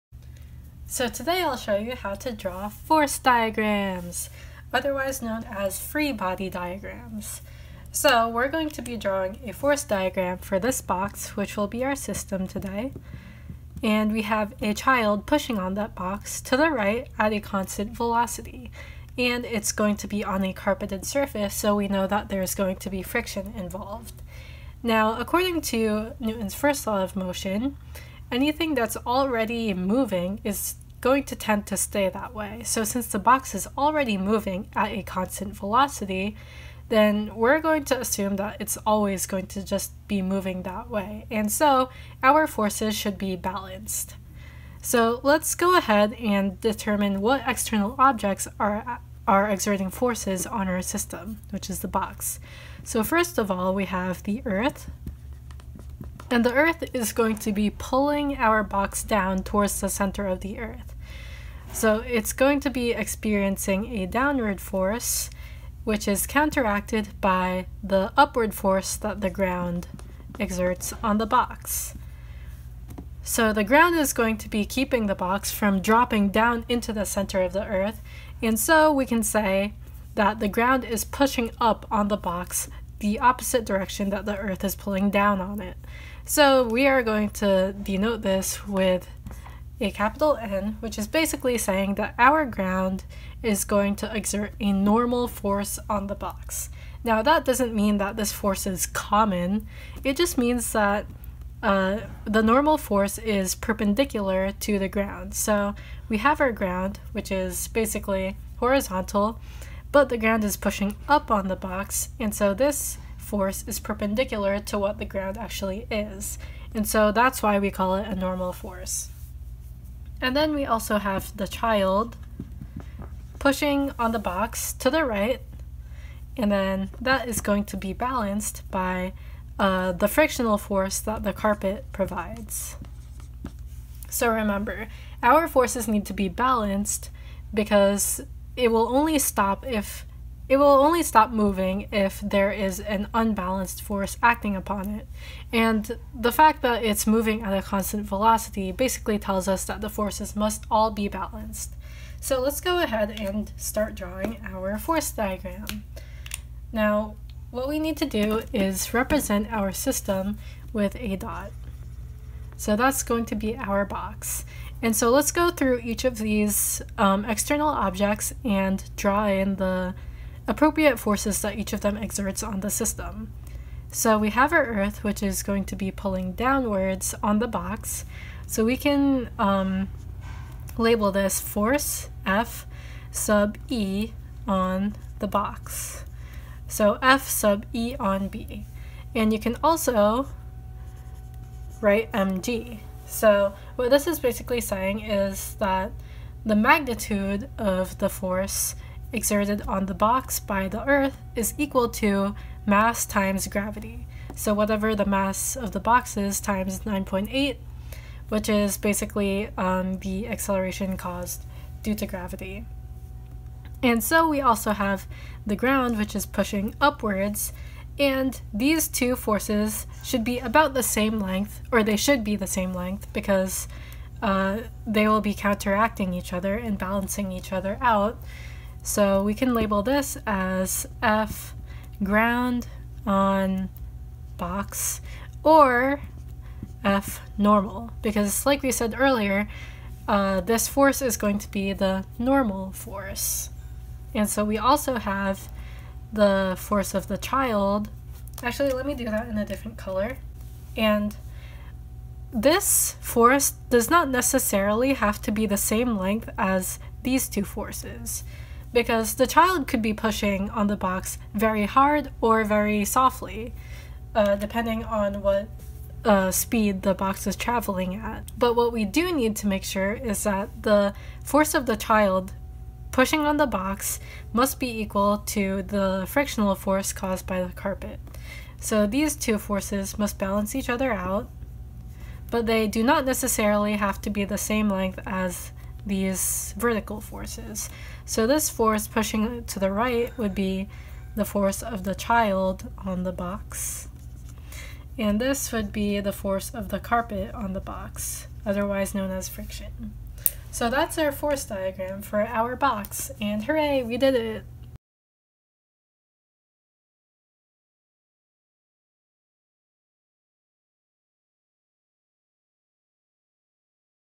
Physics: Drawing Force (Free-Body) Diagrams
A tutorial on how to draw free-body diagrams that can be applied to AP Physics and other physics courses.